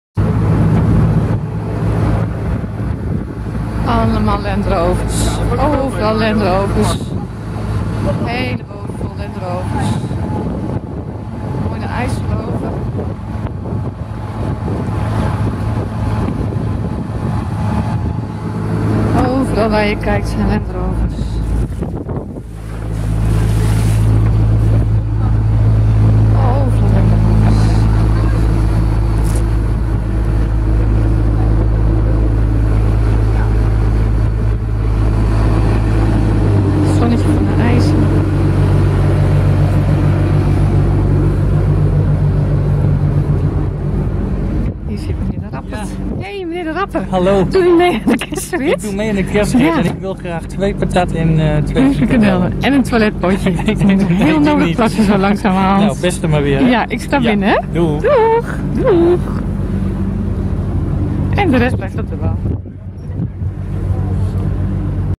2014 Nederland - Zwolle - LRCH kerstrit
Auto verkleed als een grote kerstman. We hadden dit jaar heel erg ons best gedaan om onze auto te versieren in kerstsfeer. Volgens de traditie van de club. Maar we hebben geen prijs gewonnen. Helaas. Gelukkig maakte deze perfect georganiseerde dag veel goed.